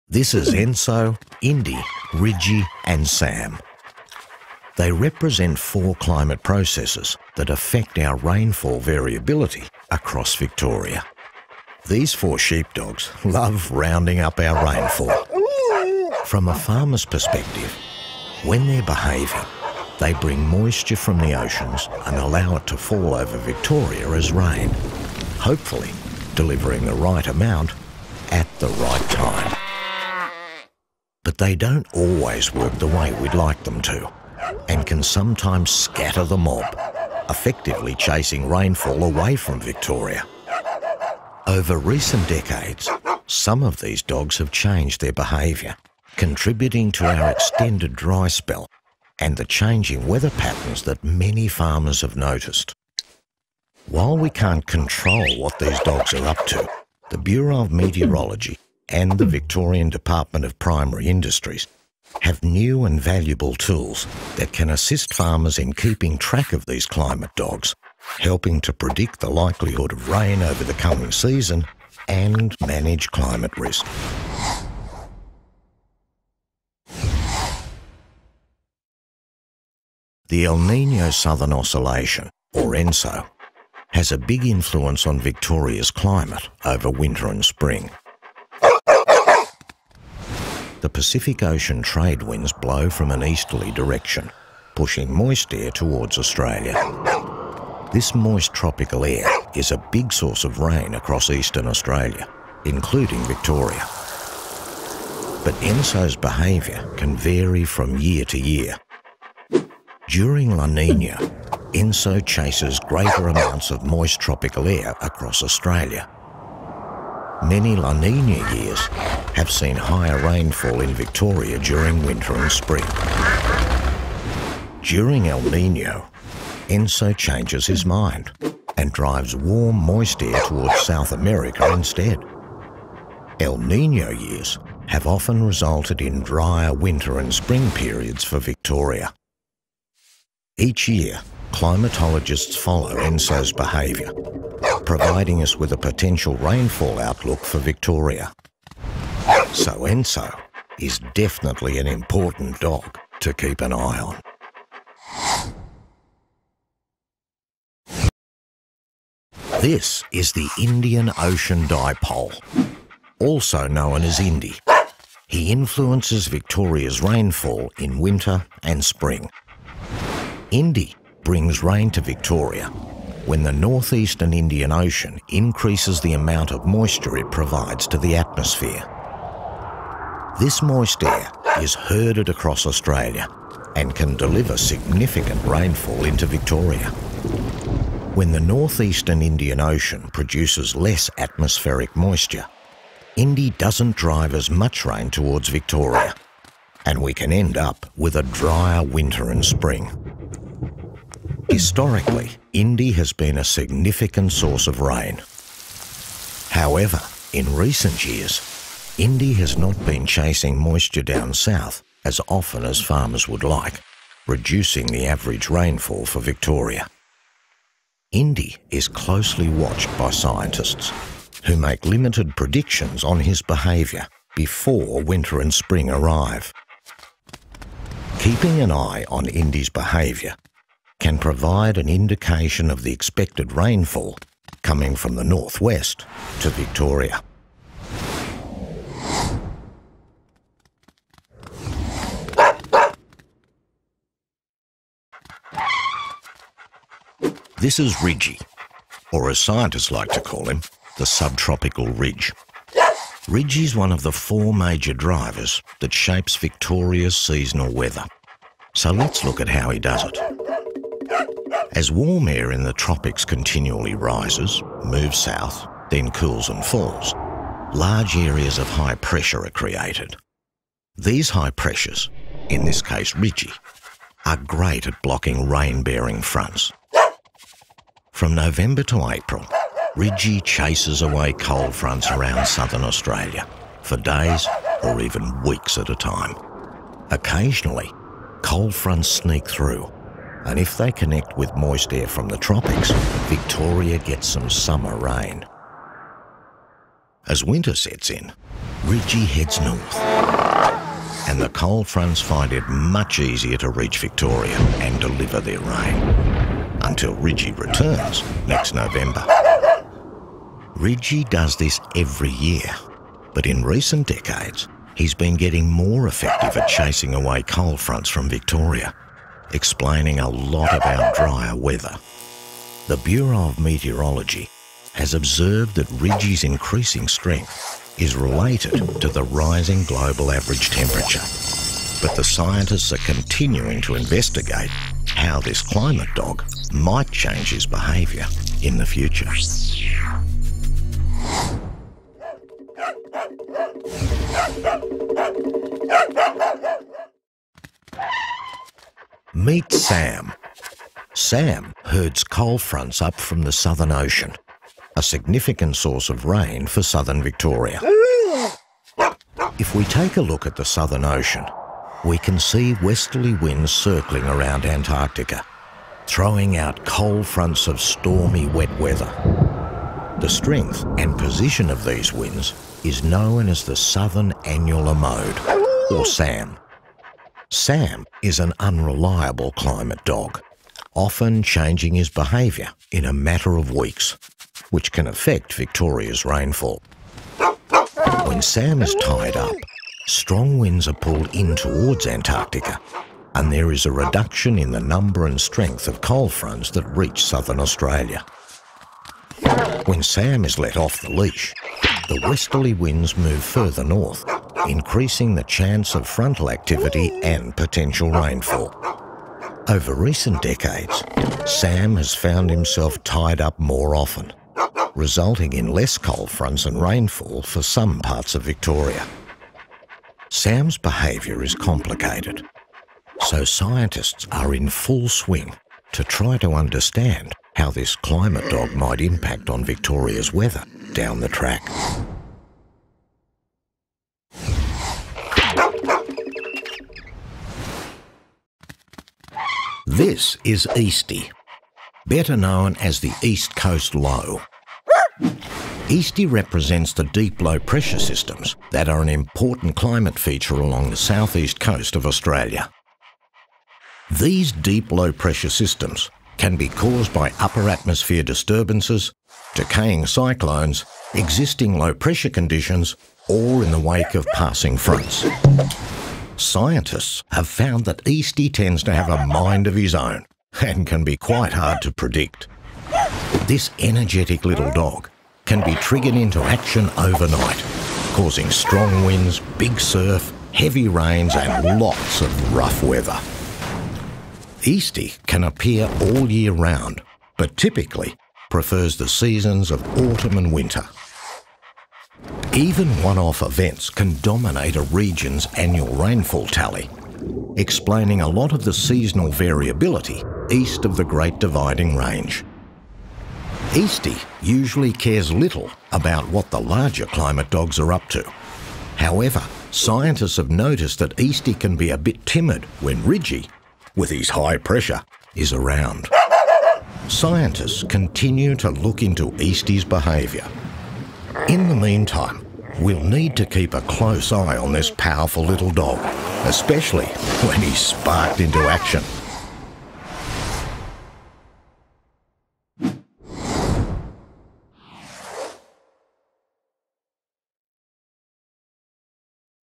Roundup of Climatedog animations
The Climatedogs animation series is an award winning series of short animated videos produced by DEPI. The animations explain what drives the climate in Victoria, and how climate drivers are changing over time.